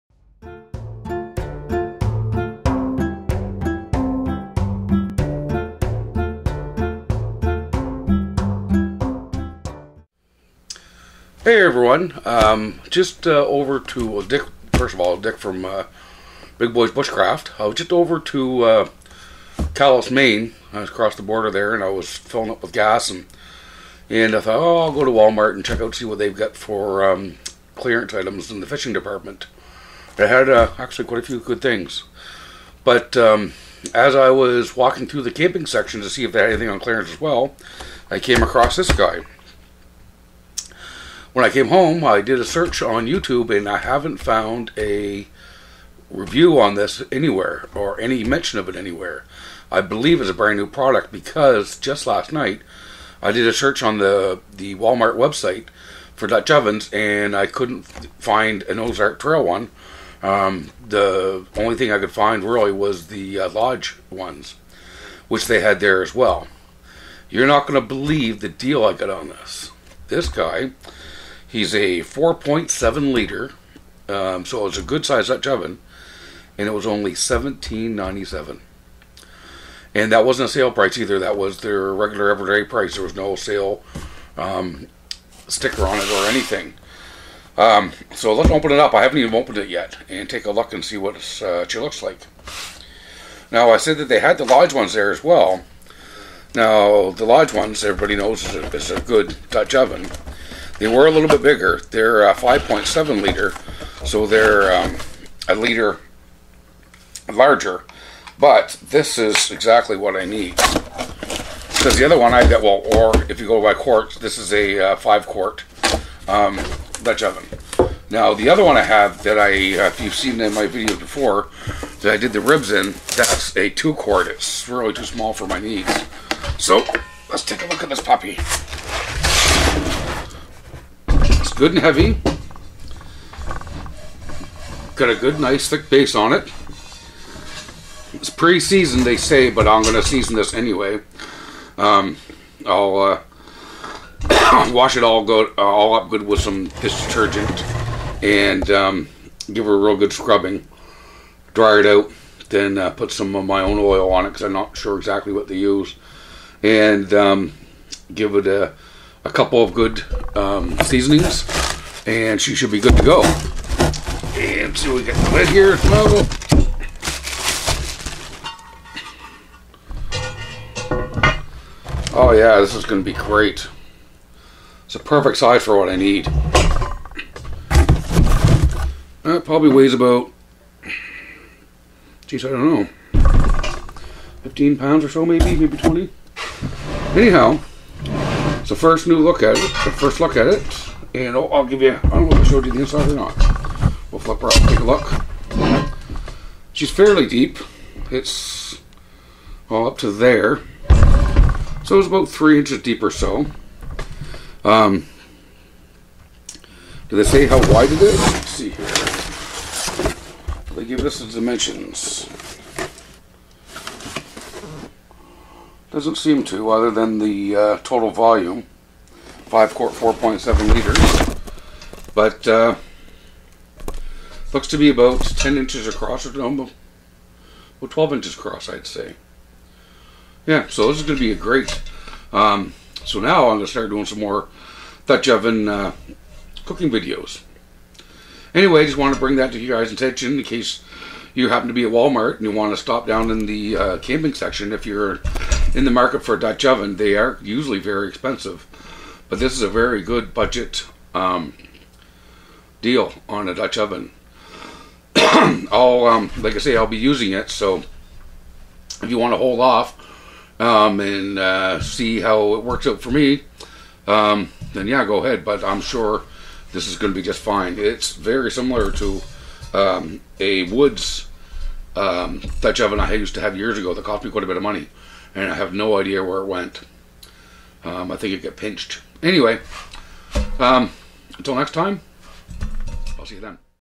Budget Dutch Oven By Ozark Trail
New item from Ozark Trail the 4.7 liter Dutch Oven that won't break the bank